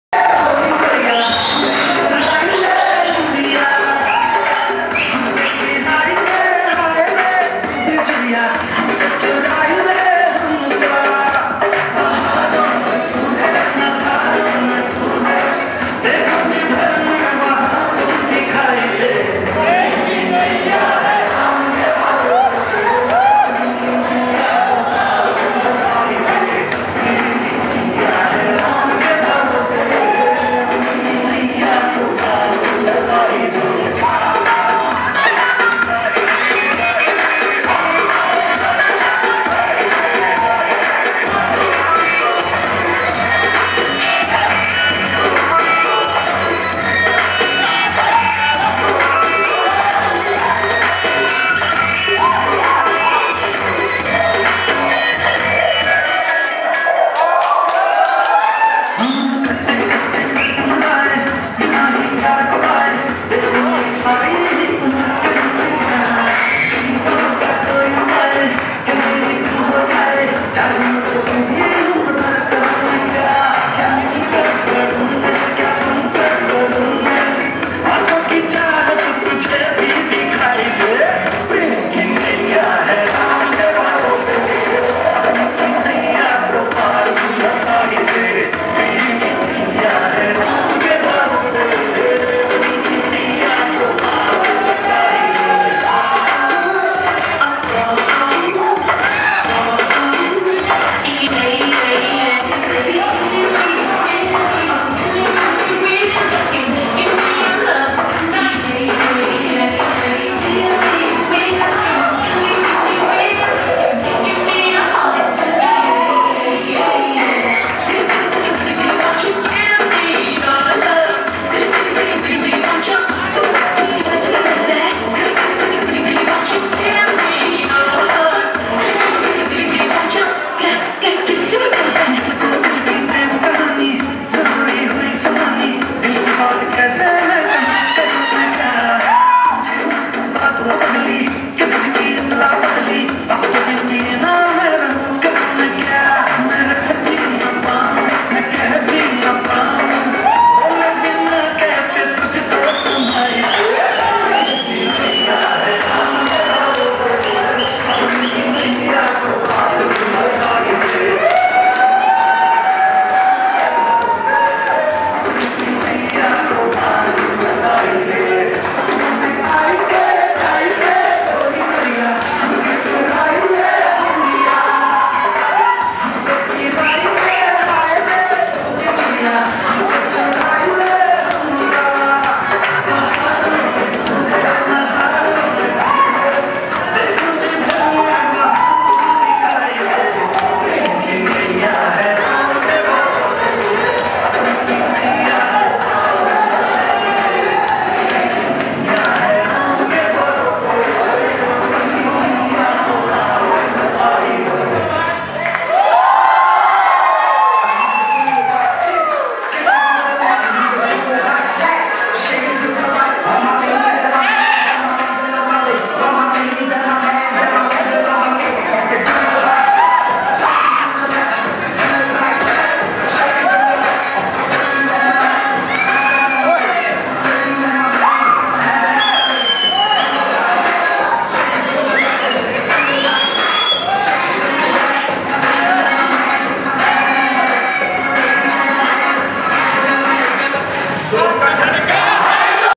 CENTRAL DEPARTMENT OF CHEMISTRY, TU (CDC-TU)
group dance by all the student of central department of chemistry on the occasion of welcome n farewel program,